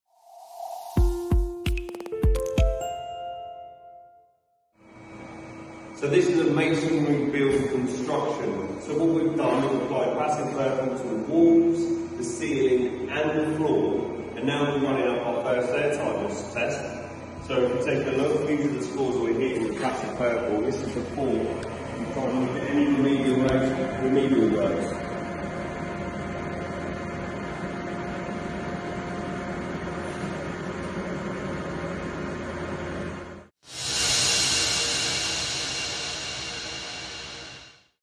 Understanding Air Testing and How Passive Purple Achieves Perfect Airtightness
Air Testing—also known as Air Tightness Testing, Air Permeability Testing, Air Leakage Testing, and Air Pressure Testing—is crucial for evaluating how much air escapes through a building’s envelope. This test determines the energy efficiency of a building and ensures it meets high-performance standards like Passivhaus.

The Passivhaus standard sets a maximum allowable air permeability of 0.6 air changes per hour at 50 Pascals (ACH50). This means that when tested at a pressure of 50 Pascals, the building should not leak more than 0.6 times its total volume of air per hour, ensuring minimal energy loss and maximum comfort.

With Passive Purple Airtight Vapour Control, achieving Passivhaus standards is made easy. Our product delivers consistent, reliable airtightness, ensuring that your building is sealed, energy-efficient, and ready to pass the strictest air permeability tests.

Ready to take your airtightness to the next level?